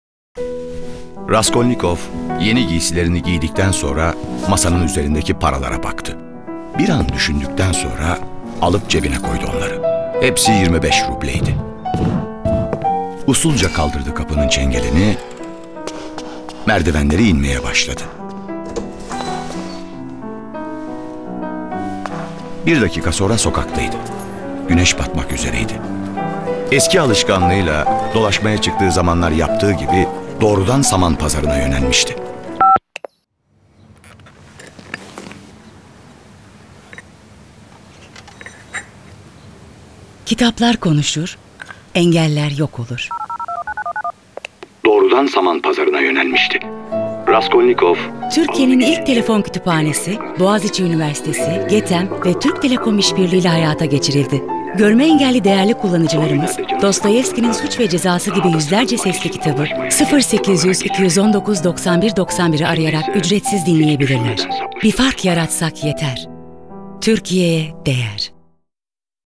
Kitaplar Konuşur, Engeller Yok Olur - Türk Telekom Telefon Kütüphanesi
Görme engellilerin telefondan dinleyebileceği bu eserler arasında şiirden romana, tarihten kişisel gelişime, deneme sınavlarından, betimlemeli filmlere 50’den fazla kategori mevcut.

Hakkımızda
Hayallerinize inanıyoruz. Hayalleriniz gerçek olsun diye, güçlü altyapımızla Türkiye’nin 81 ilindeyiz!